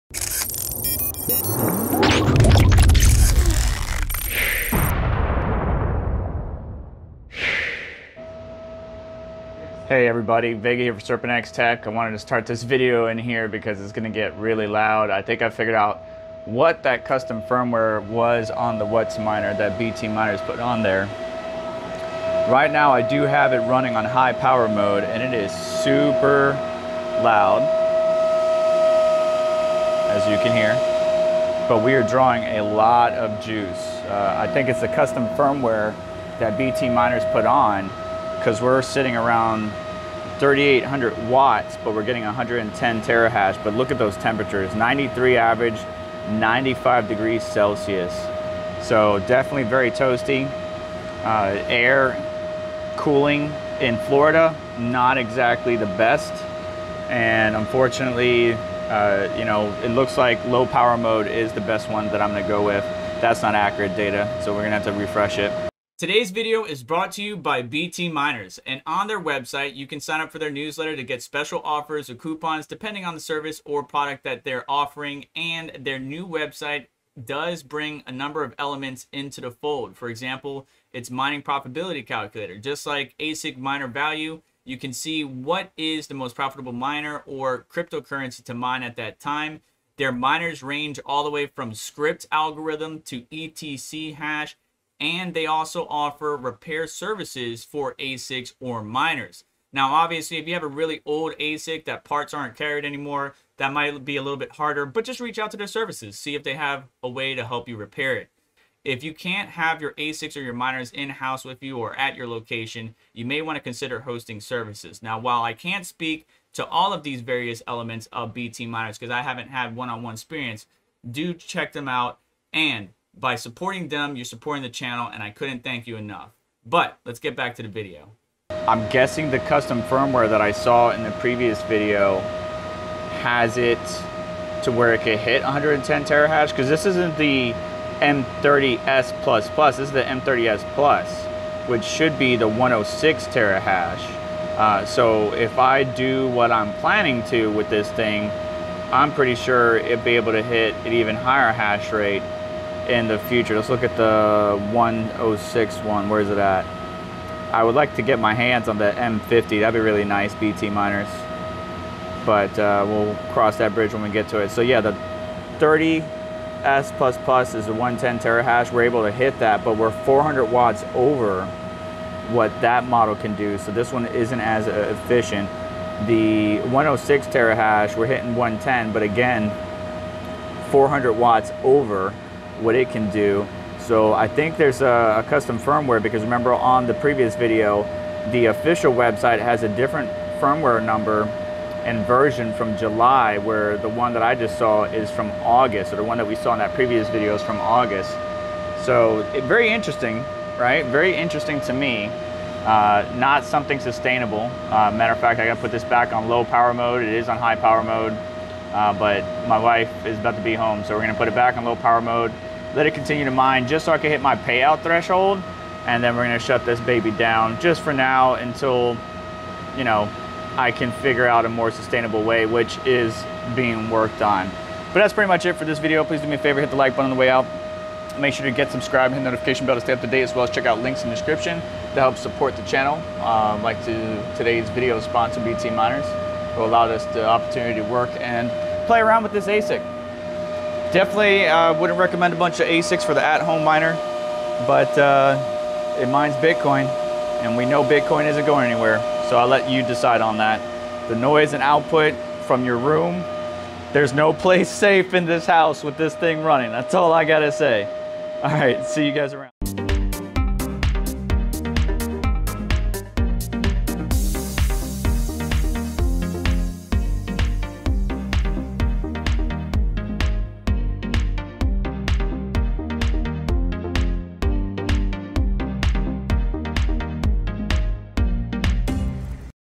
Whatsminer M30S+ from BTminers can Pull some juice
Wow, this thing can draw some power, more than what the spec sheet says... it looks like there was a custom firmware applied to this ASIC from BT Miners. Huge thanks to BT Miners for allowing me the opportunity to test and tune this Whatsminer M30S+ ASIC Bitcoin miner.